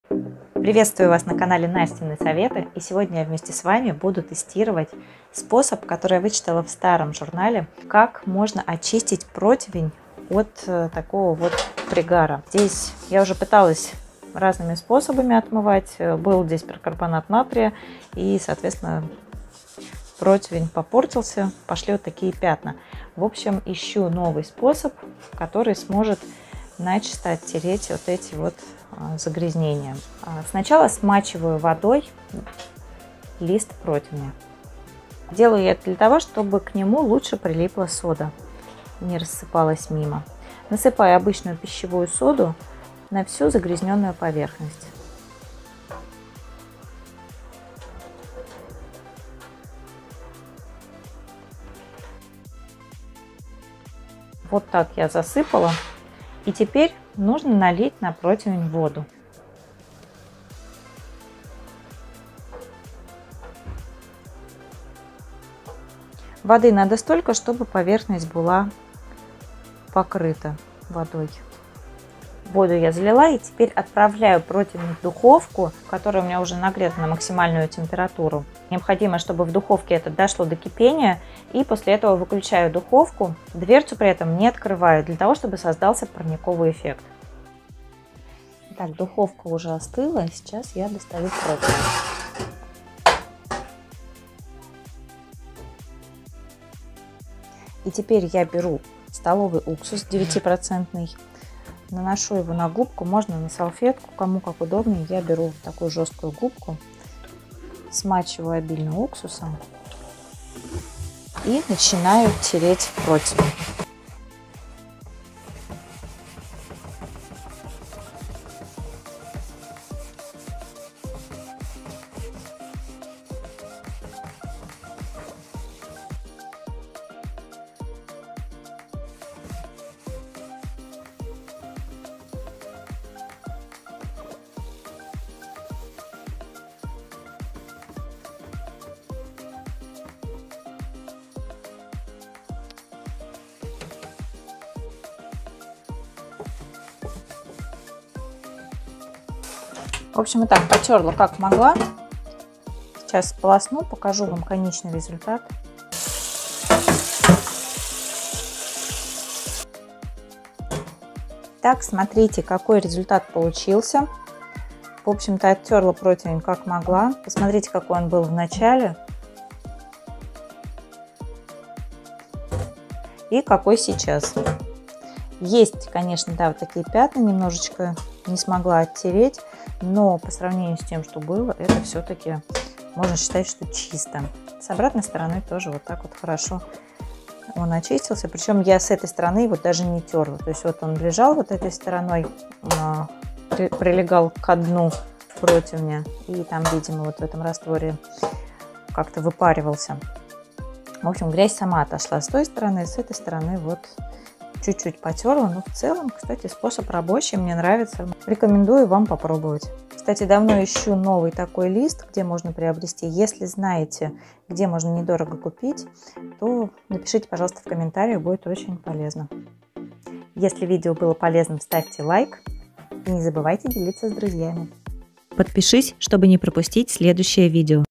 Как отмыть противень от застарелого жира и нагара: народный метод за 5 рублей
Раньше для мытья духовки и противня пользовалась едкой химией. Но теперь стараюсь выбирать бюджетные и экологичные способы. Нашла Советский рецепт, как отмыть противень и духовку от жиоа и нагара.

Рассказала о нем в видео.

Знаете другие способы, делитесь в комментариях.